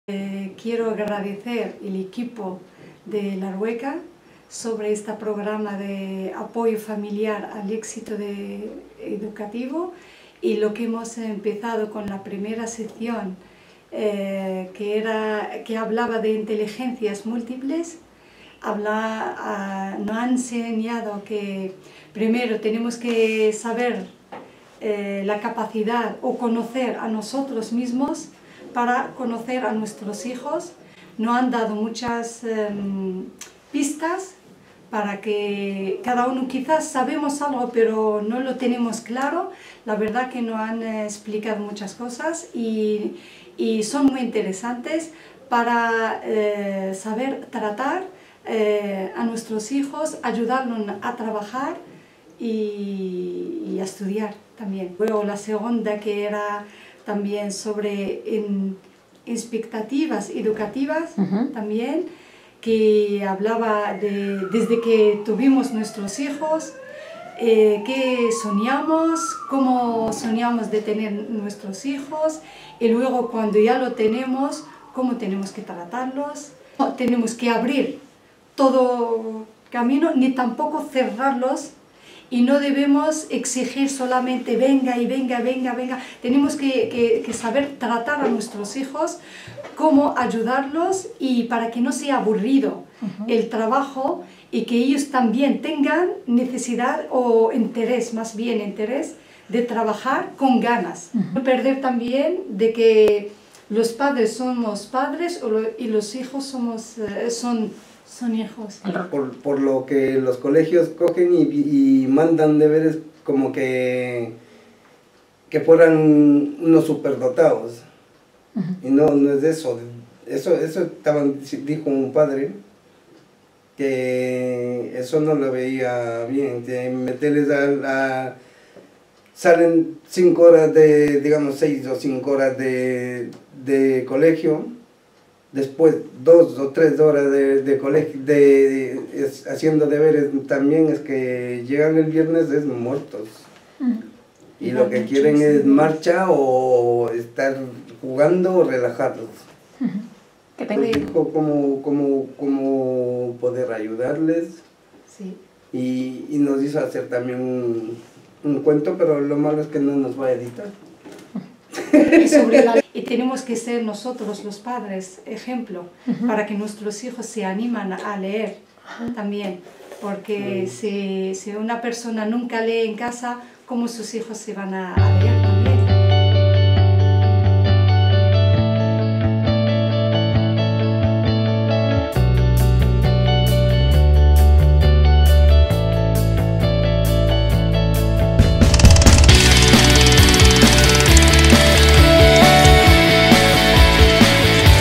Exito Escolar La Rueca
Pequeña entrevista a una madre y un padre participantes en el proyecto de Éxito Escolar desarrollado en La Rueca impulsado por la Obra Social La Caixa